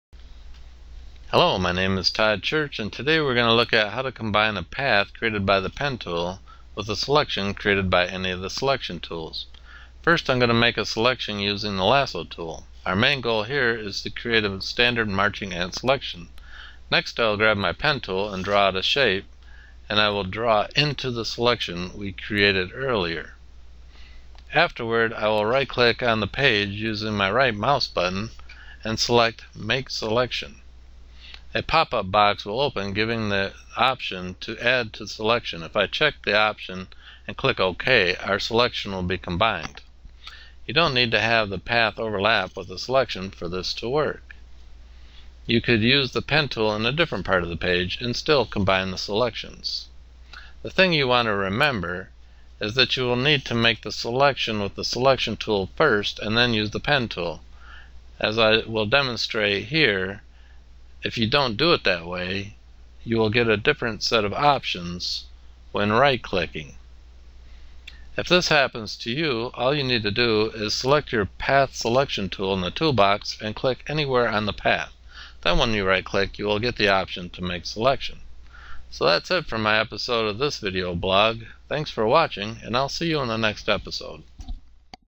Combine selections with paths in Photoshop
A photographer friend of mine was recently editing a photo in Photoshop when he created a problem. He made a selection using the lasso tool and wanted to combine it with a selection he made with the pen tool. After a little clicking in Photoshop I was able to show him how to combine those selections. This is how I did it. I hope it helps you in your photoshop adventures.